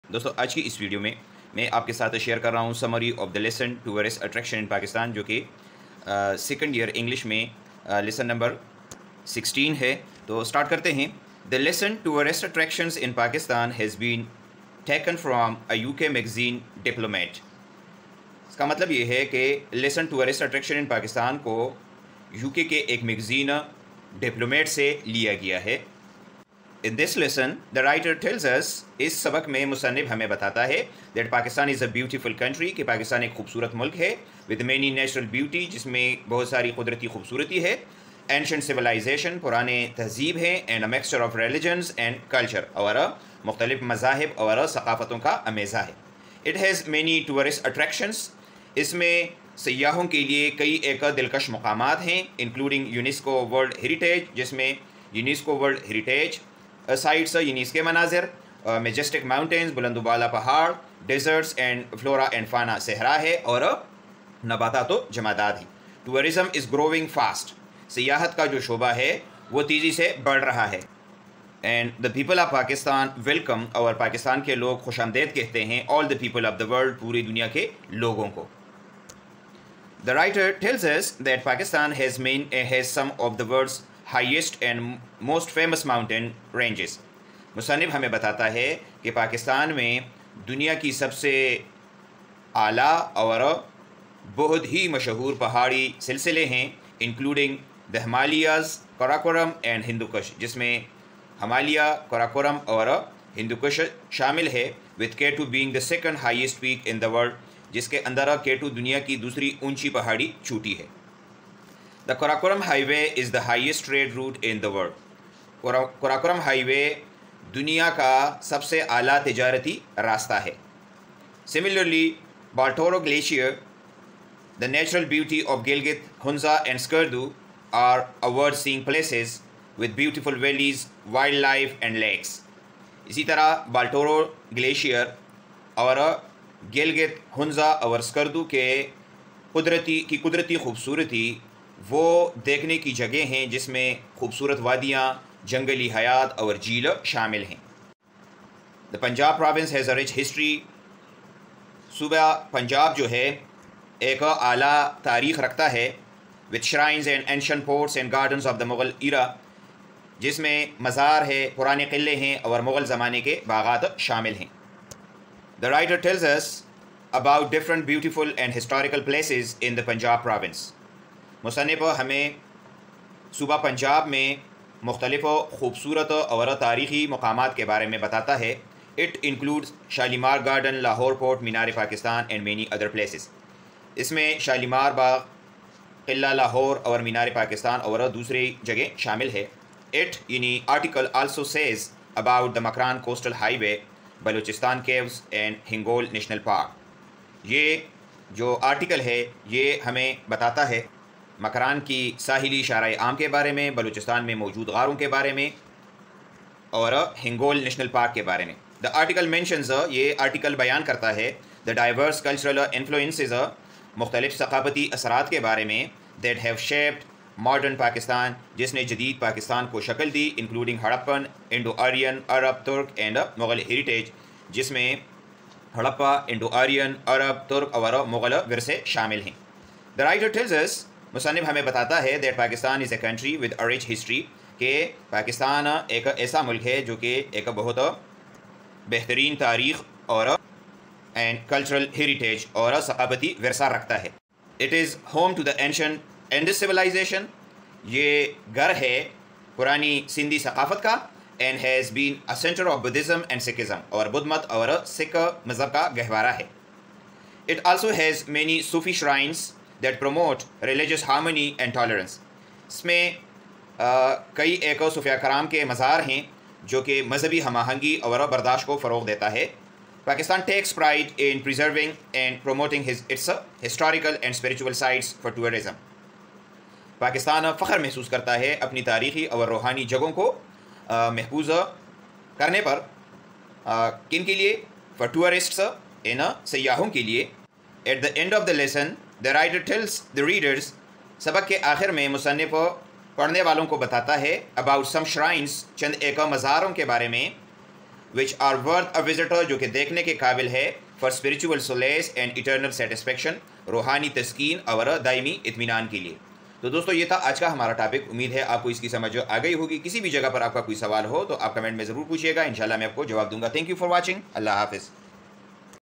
Summary || Tourist Attractions in Pakistan Summary || Summary of Tourist Attractions in Pakistan
In this video I have explained Tourist Attractions in Pakistan summary. Summary of Tourist Attractions in Pakistan in Urdu language has been explained. I hope students will enjoy this video about Summary of the lesson tourist Attractions in Pakistan.